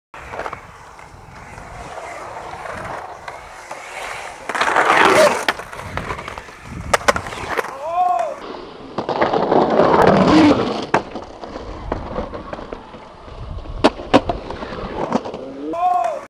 stunts and maneuvers two teaser
rich payne and keith baldassare at cocoa beach filming for stunts and ma